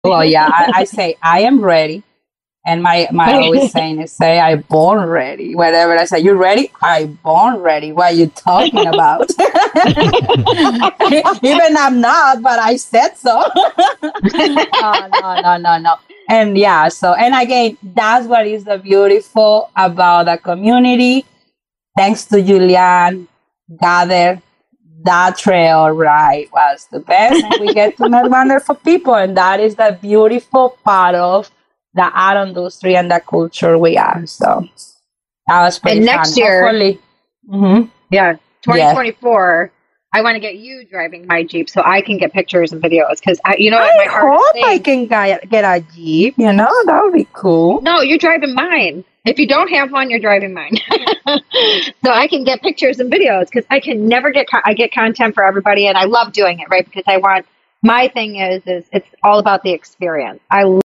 Episode 879 JTS Chic Chat with Rossy of Maxxis Tires!
Interview with Rossy of Maxxis Tires.  Rossy has been involved in motor sports her entire adult life.  She is involved with Ultra 4, Formula racing, works side by side with many athletes, Race teams, Management, and Drag racing.